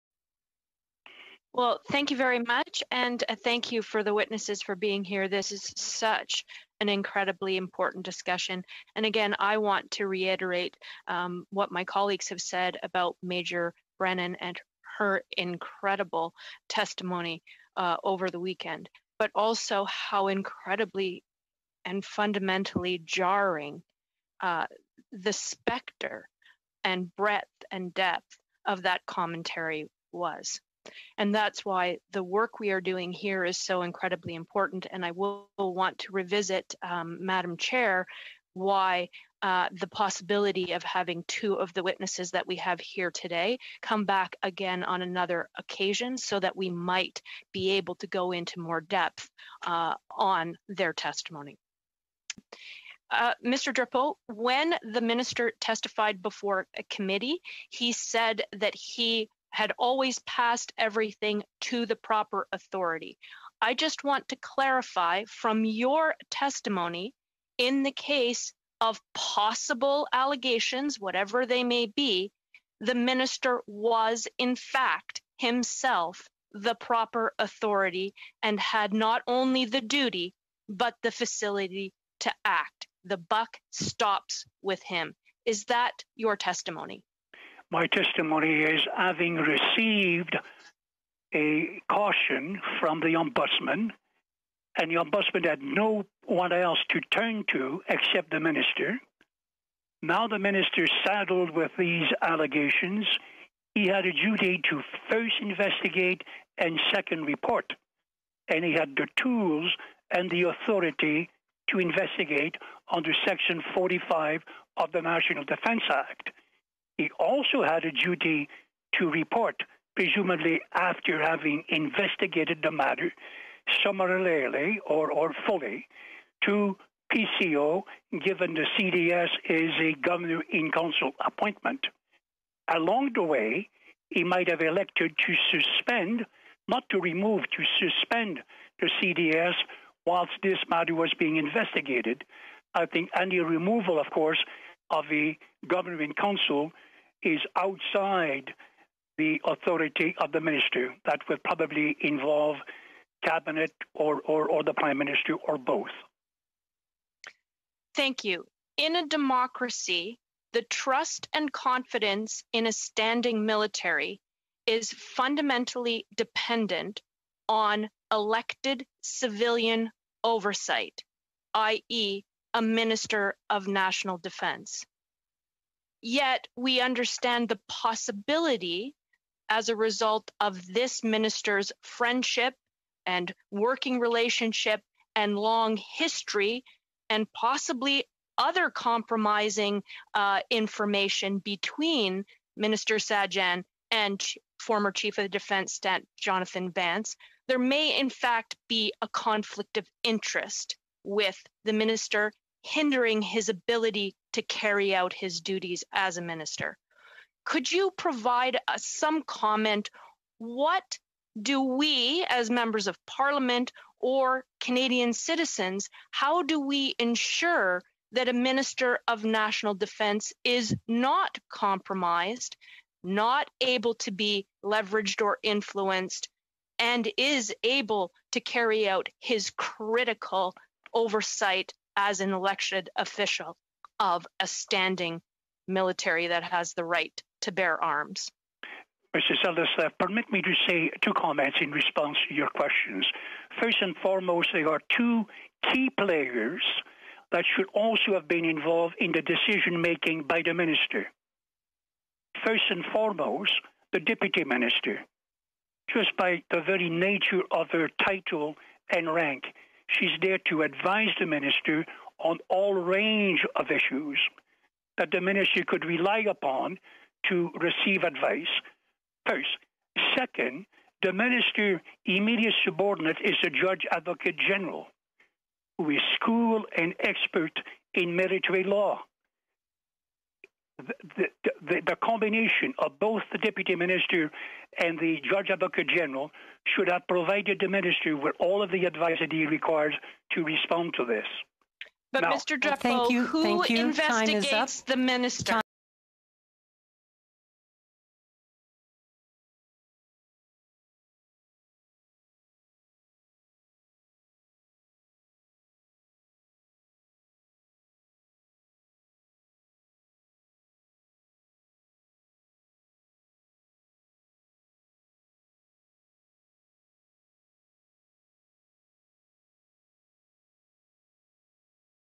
A Duty to Investigate
The Minister of National Defence, Harjit Sajjan, had a duty to investigate and suspend Canada’s former Chief of the Defence Staff, General Vance but he did nothing.  The Minister of National Defence is the appropriate authority - the buck stops with him.

Military members have lost confidence in this Minister’s ability to protect them from harm and ensure a work environment free from abuse, harassment and discrimination.

Watch my questions during committee here: